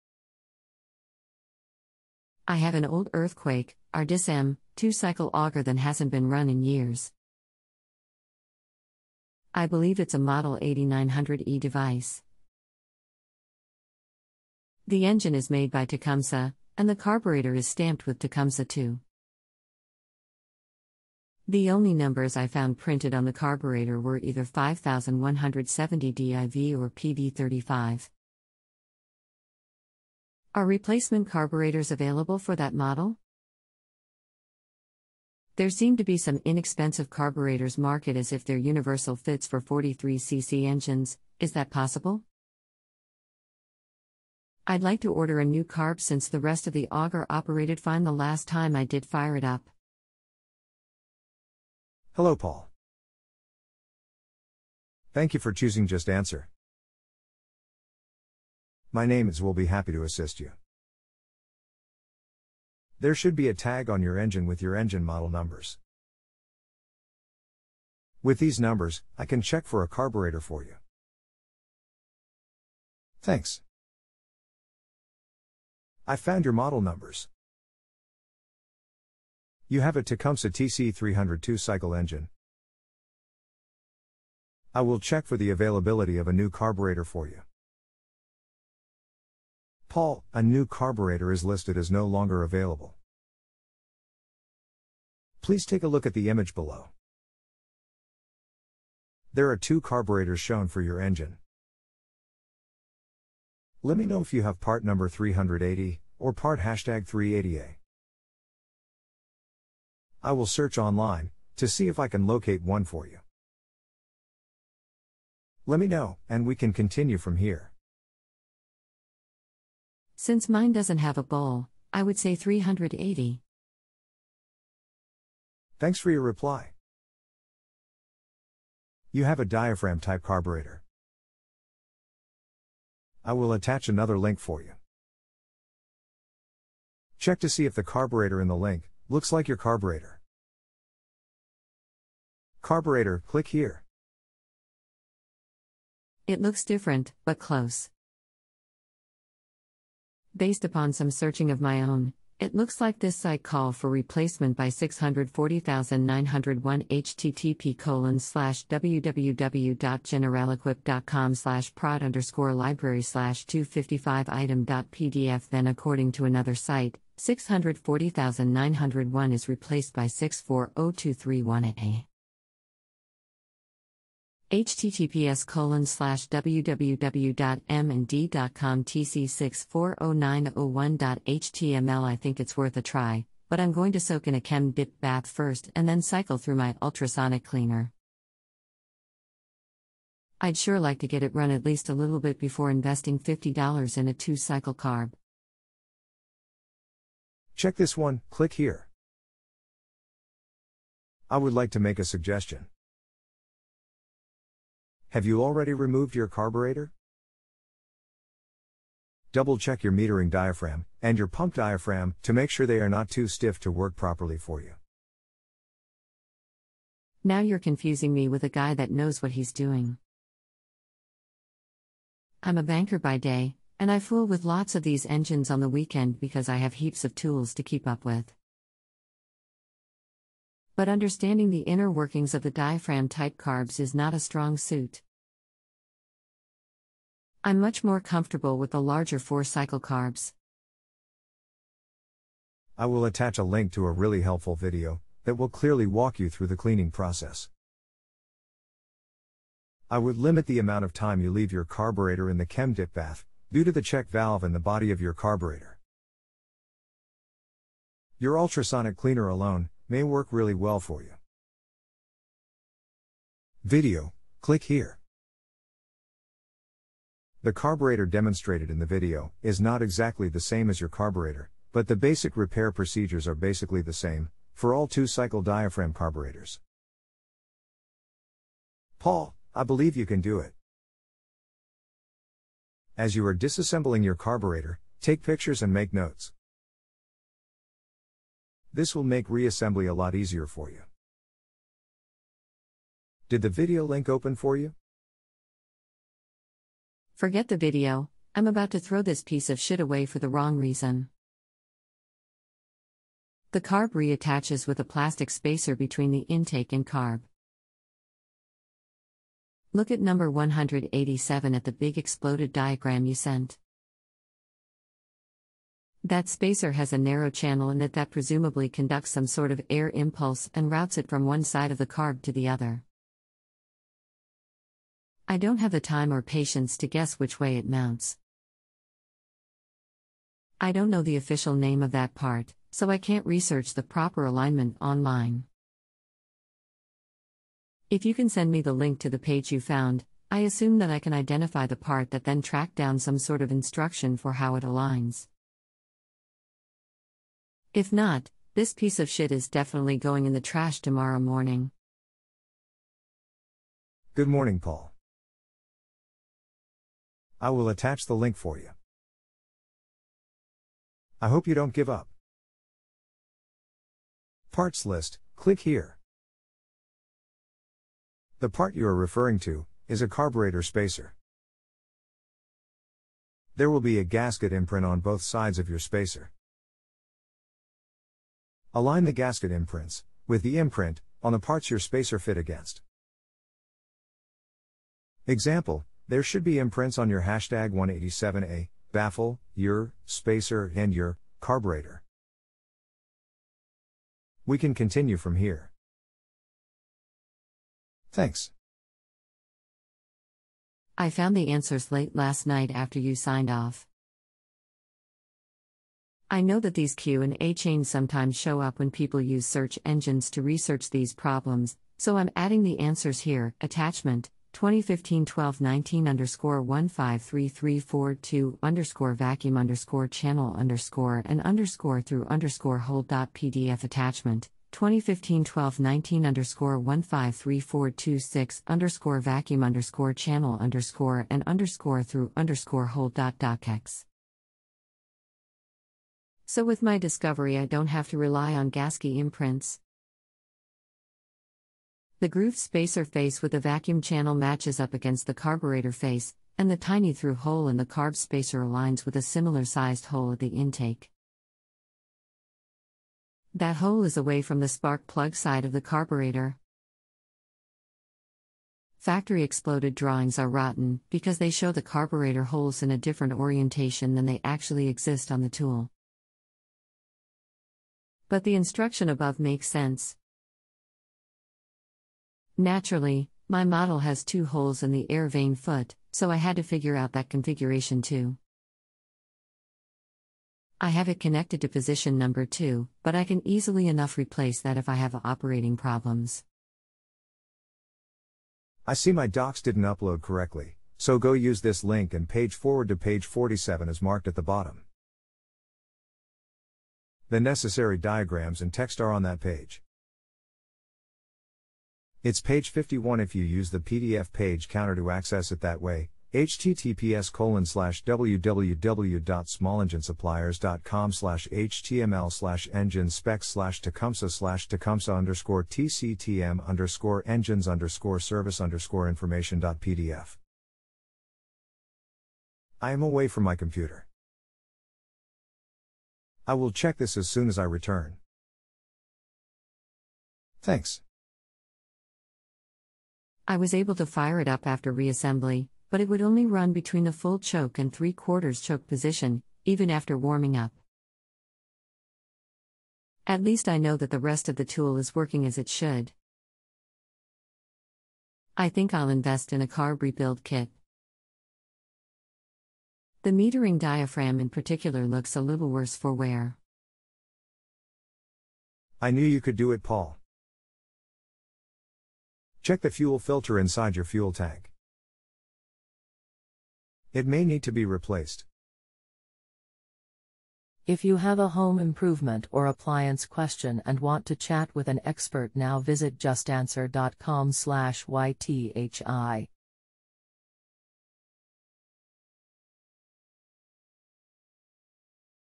I have an old Earthquake (Ardisam) 2-cycle auger than hasn't
JustAnswer Customer: I have an old Earthquake (Ardisam) 2-cycle auger than hasn't been run in years.
JustAnswer Customer: I believe it's a model 8900E device.
JustAnswer Customer: The engine is made by Tecumseh, and the carburetor is stamped with "Tecumseh" too.
JustAnswer Customer: the only numbers I found printed on the carburetor were either 5170 D I V or PV 35.
JustAnswer Customer: Are replacement carburetors available for that model?
JustAnswer Customer: There seem to be some inexpensive carburetors market as if they're universal fits for 43cc engines -- is that possible?
JustAnswer Customer: I'd like to order a new carb since the rest of the auger operated fine the last time I did fire it up.
Donald: Small Engine Technician: Hello Paul.
Donald: Small Engine Technician: Thank you for choosing Just Answer.
Donald: Small Engine Technician: My name is* will be happy to assist you.
Donald: Small Engine Technician: There should be a tag on your engine with your engine model numbers.
Donald: Small Engine Technician: With these numbers, I can check for a carburetor for you.
Donald: Small Engine Technician: Thanks!
Donald: Small Engine Technician: I found your model numbers.
Donald: Small Engine Technician: You have a Tecumseh TC300 2 Cycle Engine.
Donald: Small Engine Technician: I will check for the availability of a new carburetor for you.
Donald: Small Engine Technician: Paul, a new carburetor is listed as "No Longer Available".
Donald: Small Engine Technician: Please take a look at the image below.
Donald: Small Engine Technician: There are two carburetors shown for your engine.
Donald: Small Engine Technician: Let me know if you have part #380 , or part 380A .
Donald: Small Engine Technician: I will search online, to see if I can locate one for you.
Donald: Small Engine Technician: Let me know, and we can continue from here .
JustAnswer Customer: Since mine doesn't have a bowl, I would say 380.
Donald: Small Engine Technician: Thanks for your reply.
Donald: Small Engine Technician: You have a diaphragm type carburetor.
Donald: Small Engine Technician: I will attach another link for you.
Donald: Small Engine Technician: Check to see if the carburetor in the link, looks like your carburetor.
JustAnswer Customer: It looks different, but close.
JustAnswer Customer: Based upon some searching of my own, it looks like this site call for replacement by 640901 http:/ Then according to another site, 640901 is replaced by 640231A.
JustAnswer Customer: https:/ I think it's worth a try, but I'm going to soak in a ChemDip bath first and then cycle through my ultrasonic cleaner.
JustAnswer Customer: I'd sure like to get it run at least a little bit before investing $50 in a 2-cycle carb.
Donald: Small Engine Technician: I would like to make a suggestion.
Donald: Small Engine Technician: Have you already removed your carburetor?
Donald: Small Engine Technician: Double check your metering diaphragm, and your pump diaphragm, to make sure they are not too stiff to work properly for you,
JustAnswer Customer: Now you're confusing me with a guy that knows what he's doing.
JustAnswer Customer: I'm a banker by day, and I fool with lots of these engines on the weekend because I have heaps of tools to keep up with.
JustAnswer Customer: But understanding the inner workings of the diaphram-type carbs is not a strong suit.
JustAnswer Customer: I'm much more comfortable with the larger 4-cycle carbs.
Donald: Small Engine Technician: I will attach a link to a really helpful video, that will clearly walk you through the cleaning process.
Donald: Small Engine Technician: I would limit the amount of time you leave your carburetor in the ChemDip bath, due to the check valve in the body of your carburetor.
Donald: Small Engine Technician: Your ultrasonic cleaner alone, may work really well for you.
Donald: Small Engine Technician: The carburetor demonstrated in the video, is not exactly the same as your carburetor, but the basic repair procedures are basically the same, for all 2 cycle diaphragm carburetors.
Donald: Small Engine Technician: Paul, I believe you can do it.
Donald: Small Engine Technician: As you are dis-assembling your carburetor, take pictures and make notes.
Donald: Small Engine Technician: This will make re-assembly a lot easier for you.
Donald: Small Engine Technician: Did the video link open for you?
JustAnswer Customer: Forget the video -- I'm about to throw this piece of shit away for the wrong reason.
JustAnswer Customer: The carb reattaches with a plastic spacer between the intake and carb.
JustAnswer Customer: Look at #187 at the big exploded diagram you sent.